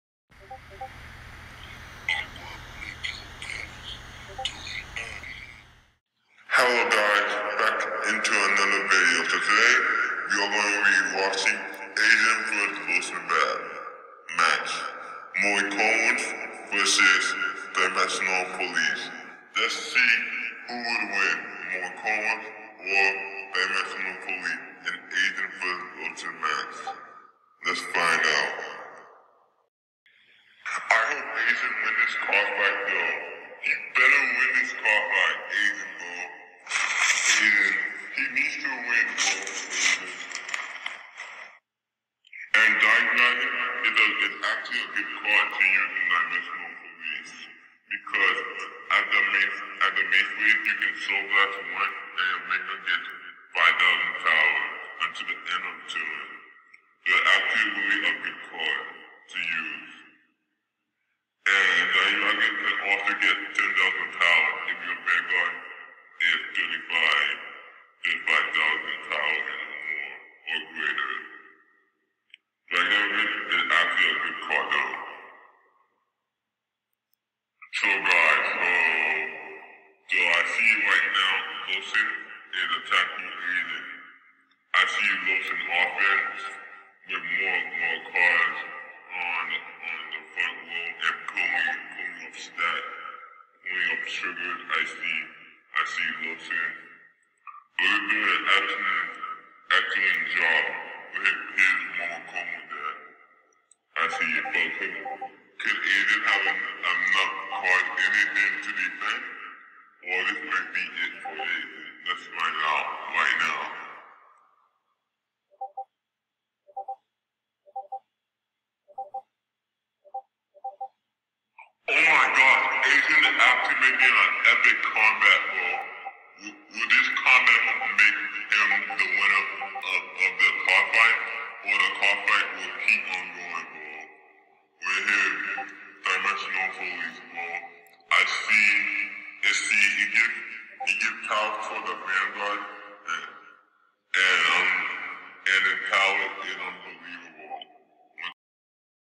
cardfight area Ultimate fight Asian vs lotion.
YOUTUBE  DISCLAIM: THIS VIDEO WAS  made for gaming purposes please don't take this off please it all about cardfighting.

Man asian made a very epic comback at the end lotion thought he haded asian on the ropes but Asian  alway makes an epic comback no matther what situation he is in.
Delete end